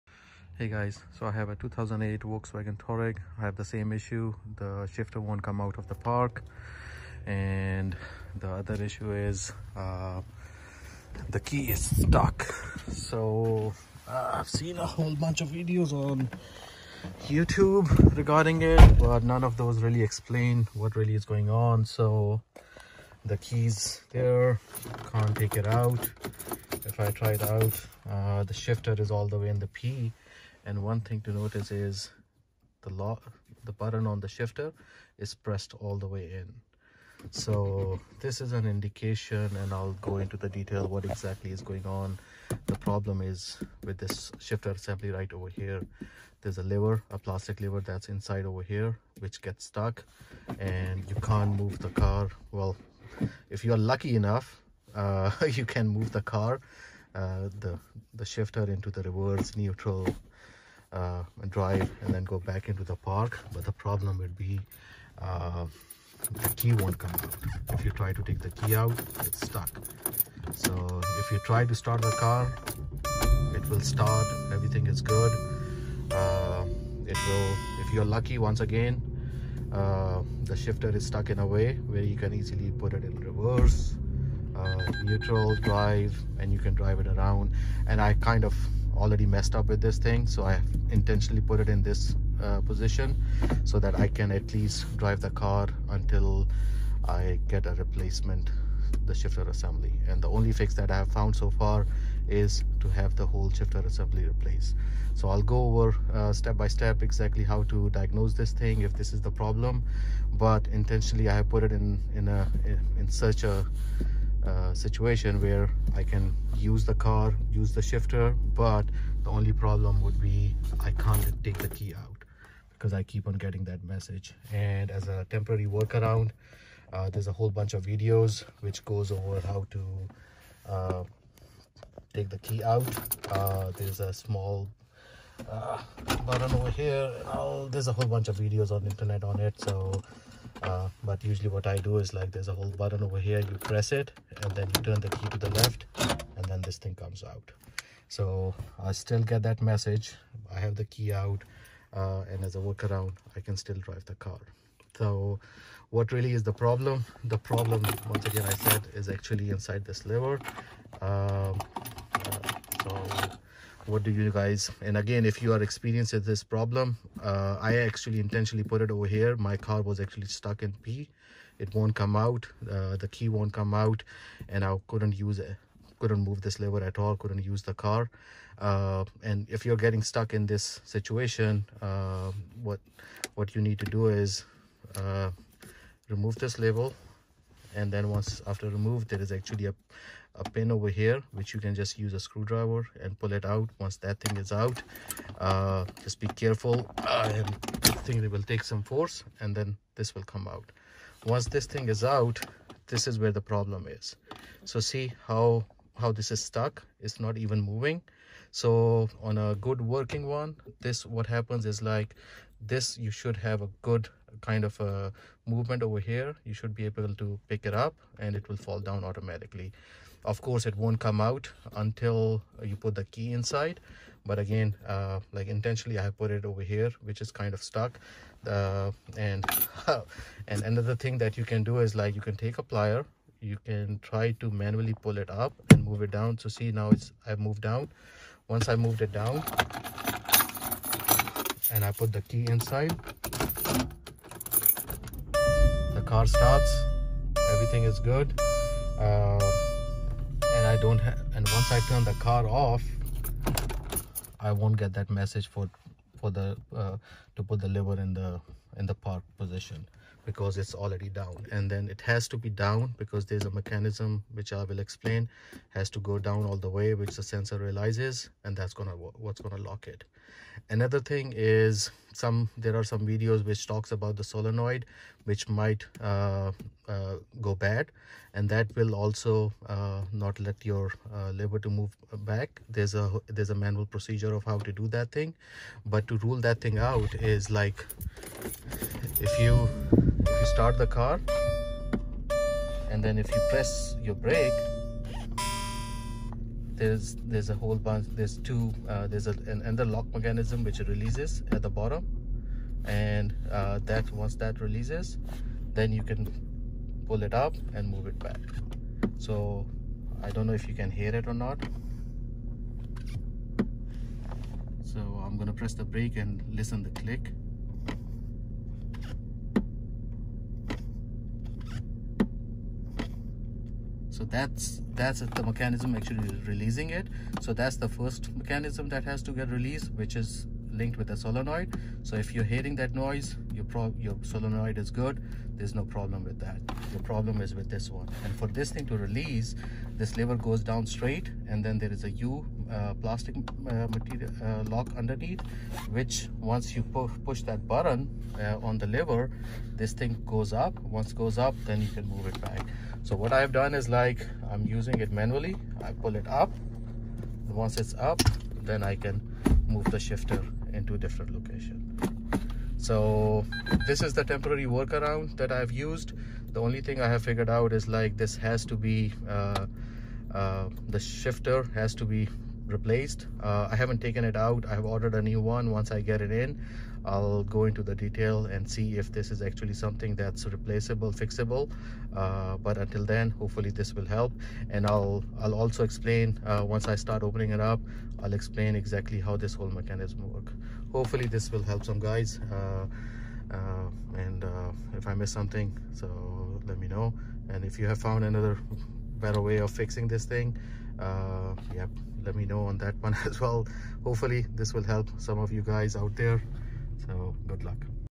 Volkswagen VW Touareg Stuck in Park Key Stuck Shifter Problem Common Problem Part 1
Volkswagen Touareg, shifter stuck. Unable to move from Park or key stuck issue. Common problem on Volkswagen